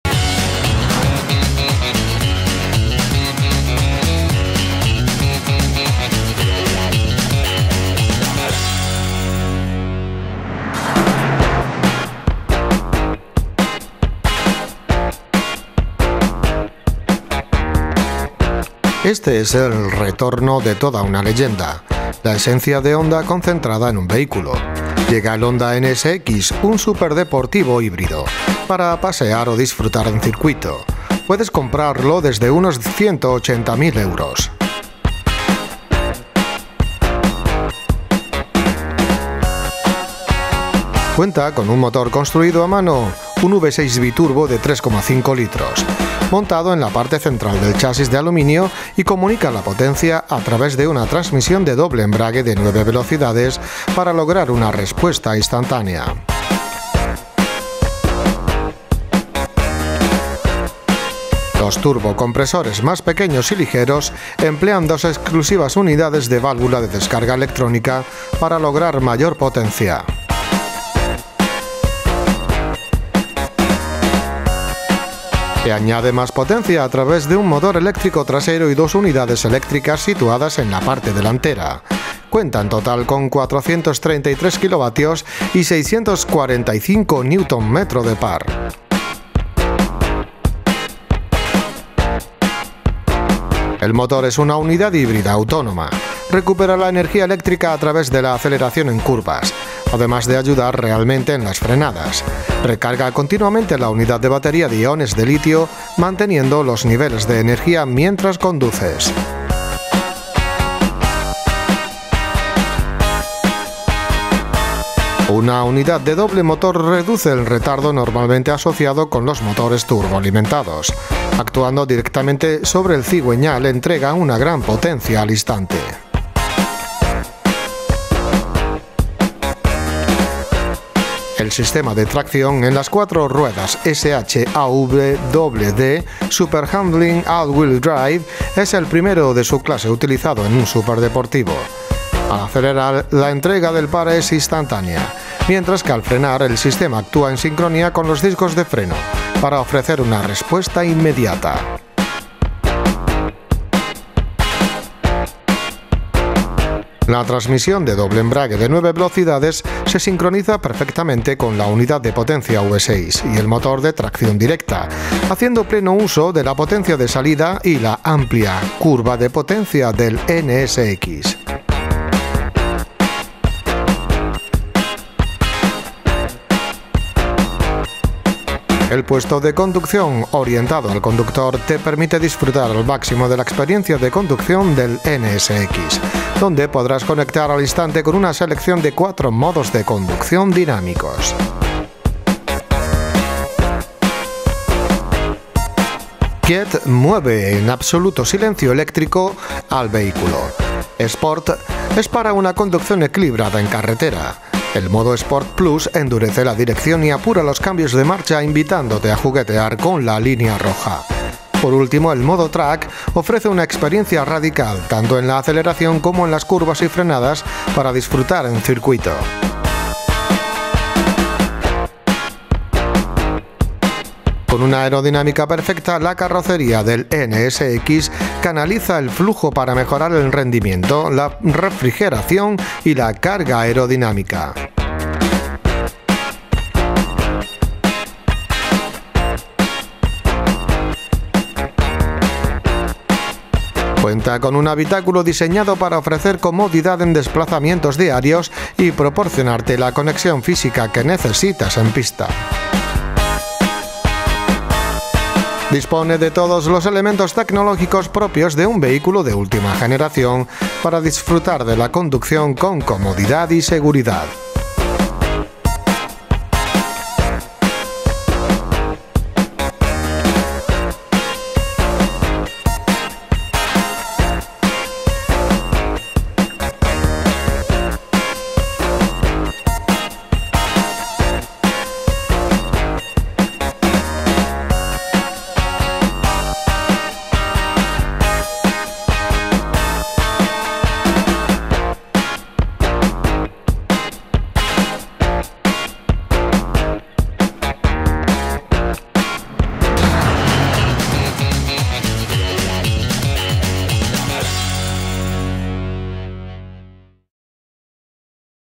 Honda NSX 2017 | Información Review Español
Todos vuestros comentarios nos ayudan a mejorar, Gracias!

Este es el retorno de toda una leyenda, la esencia de Honda concentrada en un vehículo, llega el Honda NSX, es un Superdeportivo Híbrido, para pasear o disfrutar en circuito. Puedes adquirirlo desde unos 180.000€.

Cuenta con un motor construido a mano es un V6 biturbo de 3,5 litros. Está montado en la parte central del chasis de aluminio y comunica la potencia a través de una transmisión de doble embrague de nueve velocidades para lograr una respuesta instantánea.

Los turbocompresores, más pequeños y ligeros, emplean dos exclusivas unidades de válvula de descarga electrónica para lograr mayor potencia.

Se añade más potencia a través de un motor eléctrico trasero y dos unidades eléctricas situadas en la parte delantera. Cuenta en total con 433Kw y 645 Newton Metro de par.

El motor es una unidad híbrida autónoma. Recupera la energía eléctrica a través de la aceleración en curvas, además de ayudar realmente en las frenadas. Recarga continuamente la unidad de batería de iones de litio, manteniendo los niveles de energía mientras conduces.

Una unidad de doble motor reduce el retardo normalmente asociado con los motores turboalimentados. Actuando directamente sobre el cigüeñal, entrega una gran potencia al instante.

El sistema de tracción en las cuatro ruedas SH-AWD (Super Handling All-Wheel Drive) es el primero de su clase utilizado en un superdeportivo. Al acelerar la entrega del par es instantánea, mientras que al frenar, el sistema actúa en sincronía con los discos de freno para ofrecer una respuesta inmediata.

La transmisión de doble embrague de nueve velocidades se sincroniza perfectamente con la unidad de potencia V6 y el motor de tracción directa, haciendo pleno uso de la potencia de salida y la amplia curva de potencia del NSX.

El puesto de conducción orientado al conductor te permite disfrutar al máximo de la experiencia de conducción del NSX, donde te podrás conectar al instante con una selección de cuatro modos de conducción dinámicos.

Quiet, Muévete en absoluto silencio eléctrico si lo deseas. Sport, para una conducción equilibrada en carretera. El modo Sport+ endurece la dirección y apura los cambios de marcha invitándote a juguetear con la línea roja. Por último el modo Track ofrece una experiencia radical tanto en la aceleración como en las curvas y frenadas, para disfrutar en circuito.

Con una aerodinámica perfecta, la carrocería del NSX canaliza el flujo para mejorar el rendimiento, la refrigeración y la carga aerodinámica.

Cuenta con un habitáculo diseñado para ofrecer comodidad en desplazamientos diarios y proporcionarte la conexión física que necesitas en pista.

Dispone de todos los elementos tecnológicos propios de un vehículo de última generación para disfrutar de la conducción con comodidad y seguridad.